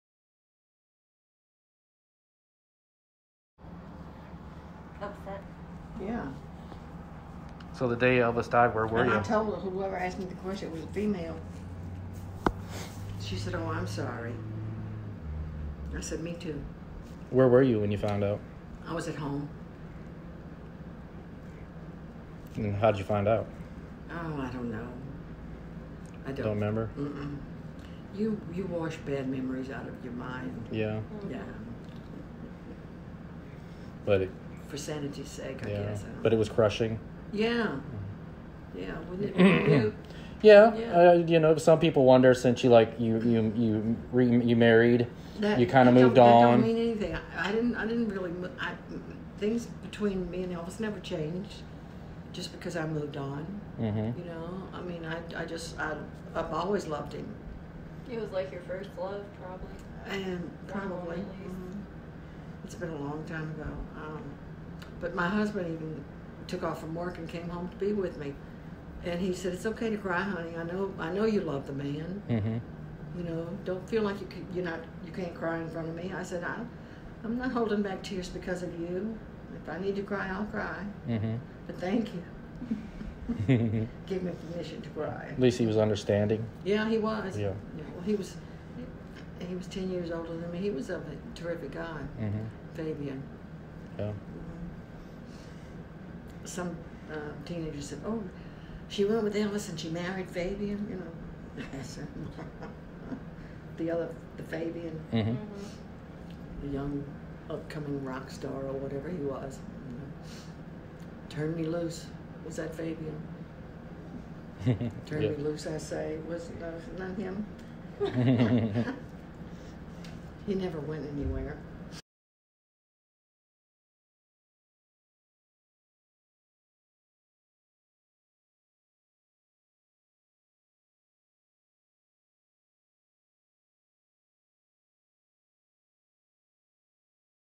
MMP June Juanico Interview Talking About The Day Elvis Died.
Interview was conducted on 29 July 2021.   June is an MMP Music Award winner and Hall of Fame Member.